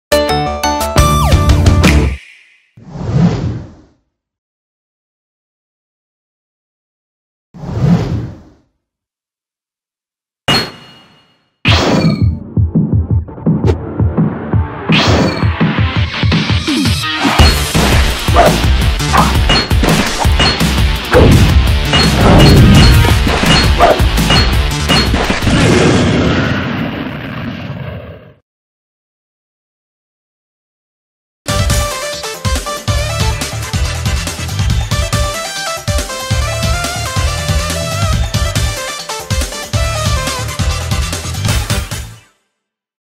Shadow in BREAK THE TARGETS - Animation
In my entry for Break the Targets 3 Collab, Shadow the ultimate life form fails to break all the targets...He does a cool job of trying though!

Find Me in Some Other Places!